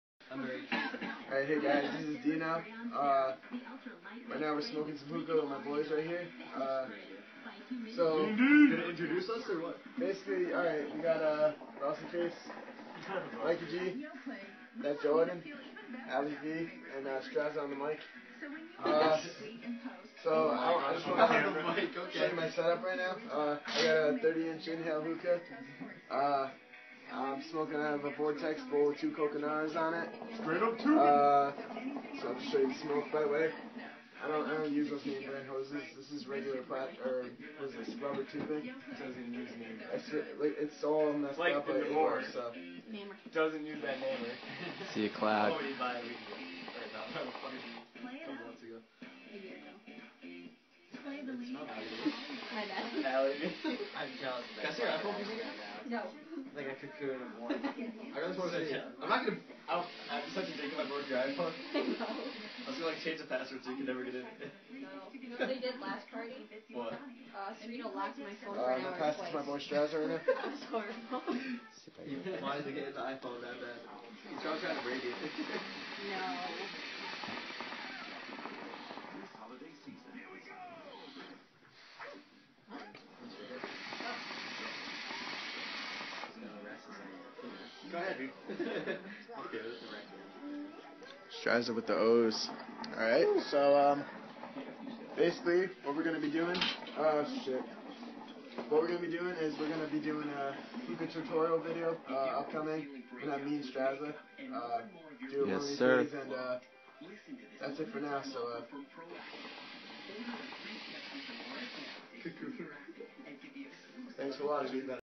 Intro Video - Dean's Hookah Setup
Decided to kick off this channel by introducing some of our friends you may see in this channel and Dean's hookah setup!